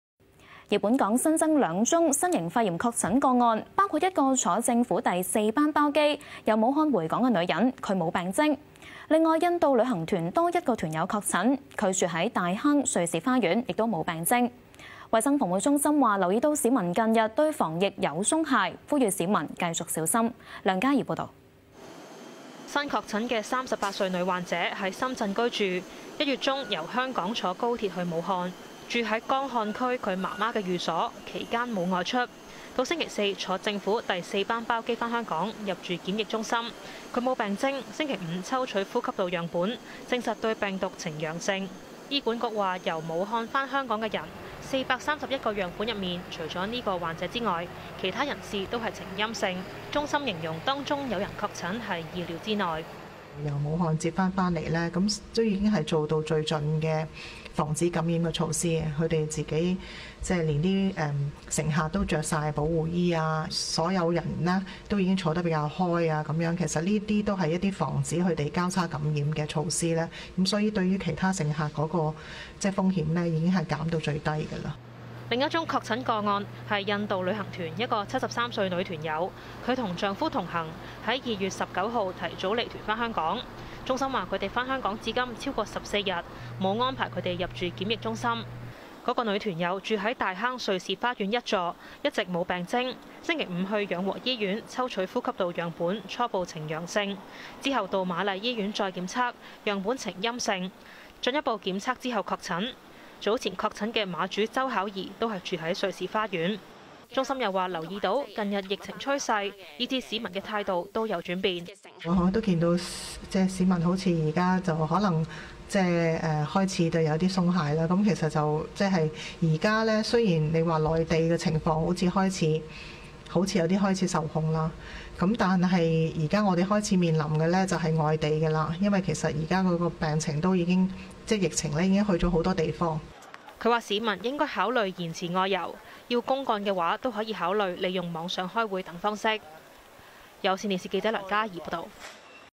新型肺炎 - 全日新增兩宗確診；一人由武漢回港 另一為印度旅行團團友（更新版） - 20200307 - 有線新聞 i-Cable News
【新型肺炎．全日新增兩宗確診】
【一人由武漢回港　另一為印度旅行團團友】
【更新版】

本港新增兩宗新型肺炎確診個案其中一人乘坐政府第四班包機，由武漢回港，她沒有病徵。另一宗是印度旅行團一名團友，她亦沒有病徵，住大坑瑞士花園。

衞生防護中心說留意到，市民近日對防疫有鬆懈，呼籲市民繼續小心。